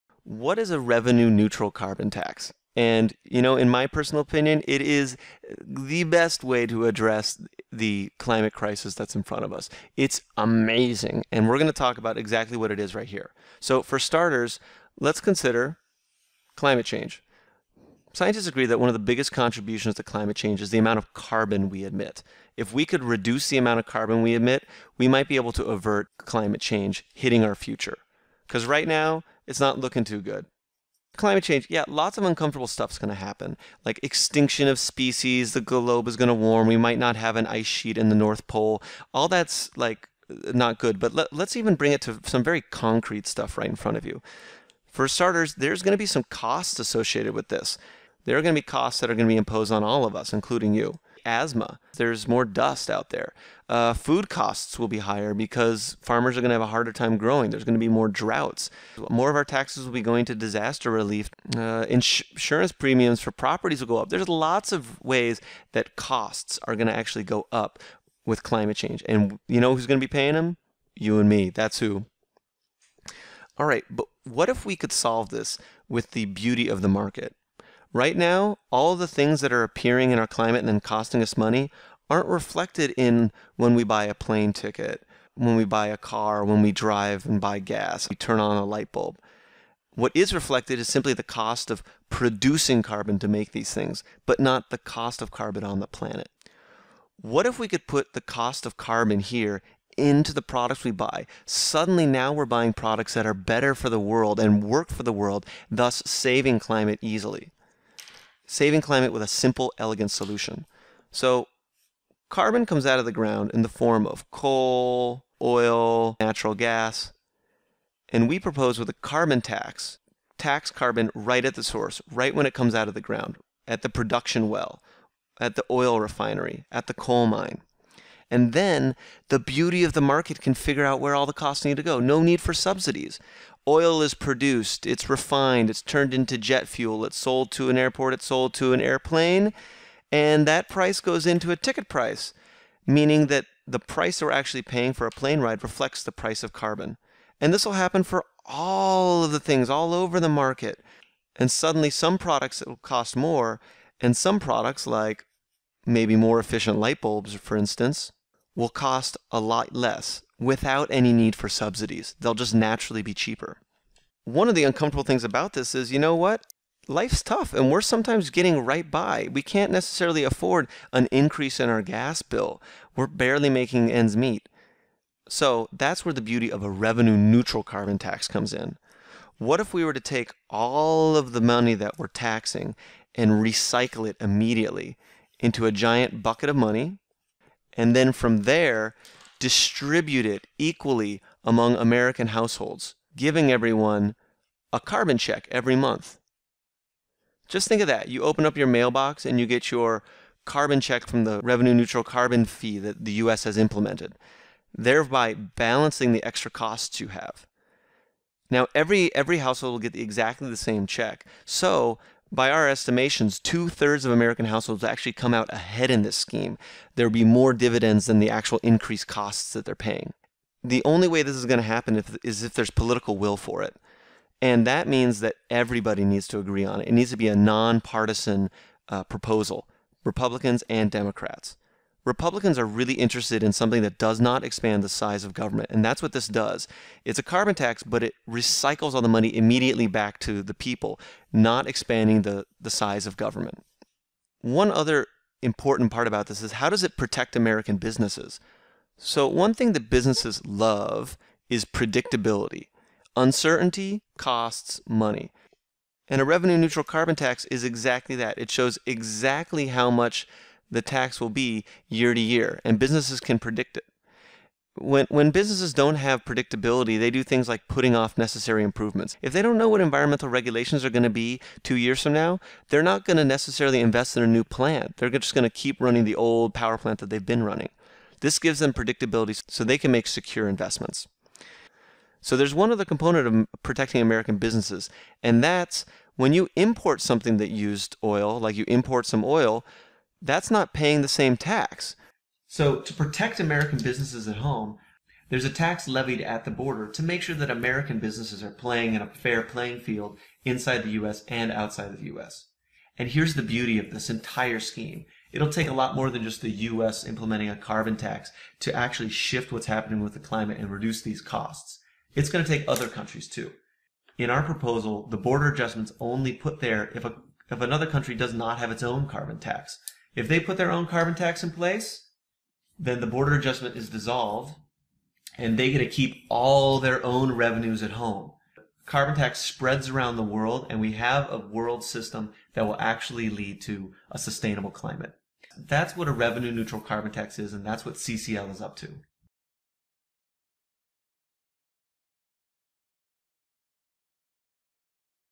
What is a Revenue Neutral Carbon Tax?
A Revenue Neutral Carbon Tax, or Carbon Fee & Dividends Plan, is a market-based and politically feasible response to Climate Change.